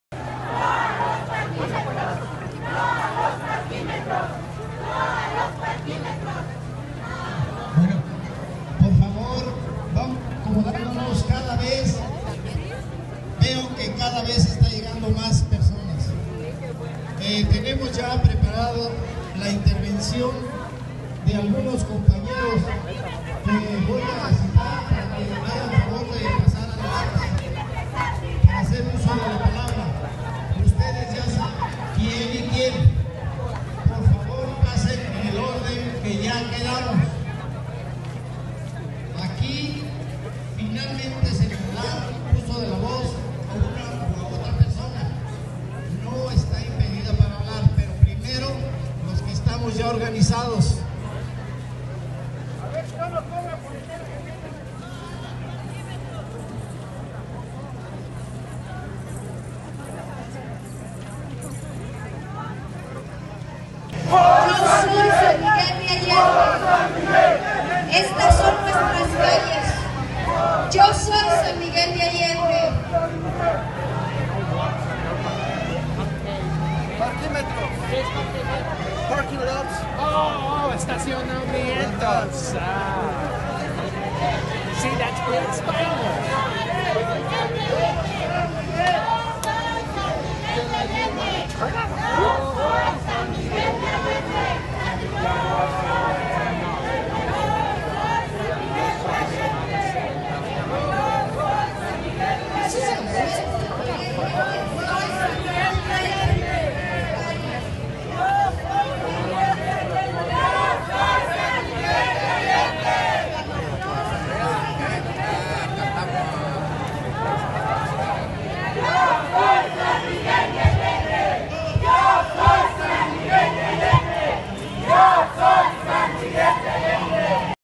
“No a los parquímetros” en San Miguel de Allende
Ciudadanos marchan y gritan consignas contra el proyecto del gobierno municipal.